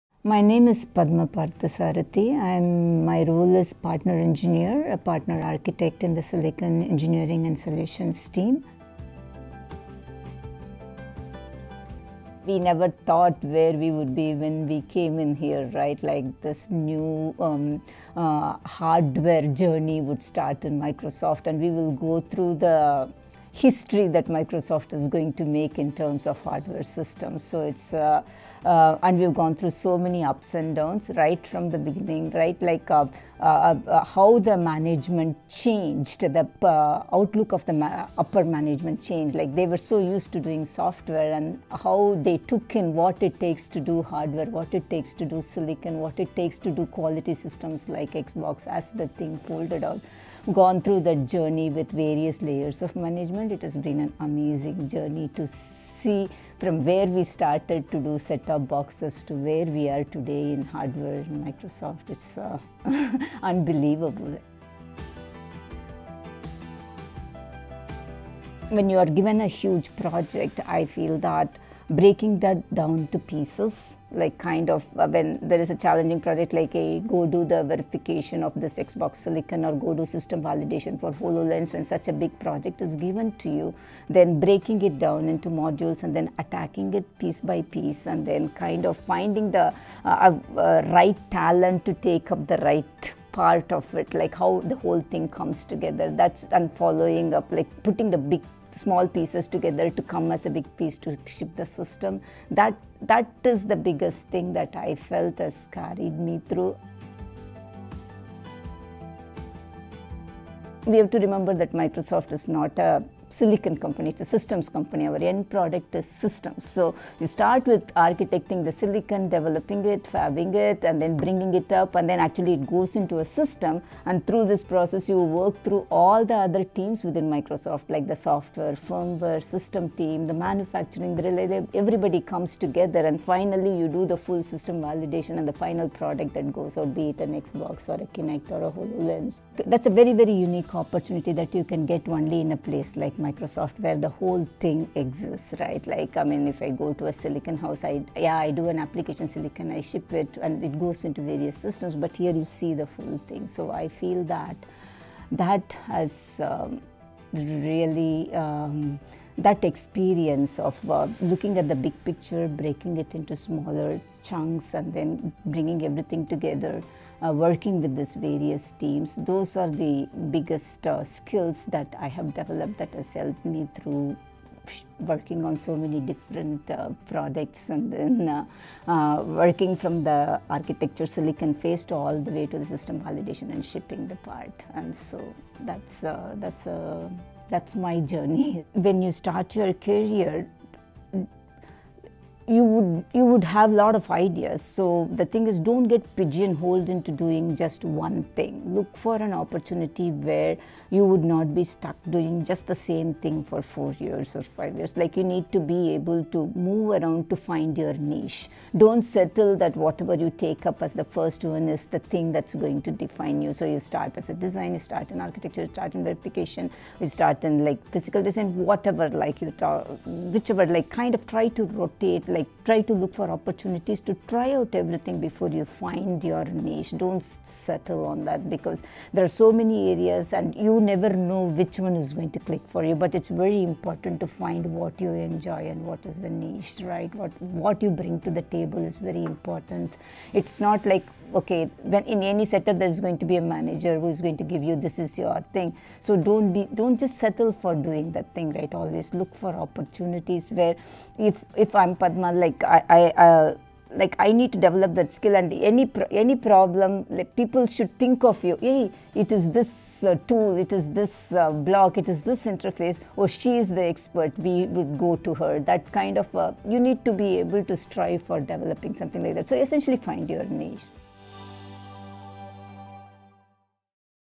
Padma Parthasarathy, Partner Design Verification Engineer
Microsoft women innovating in cloud hardware — Padma Parthasarathy, Partner Design Verification Engineer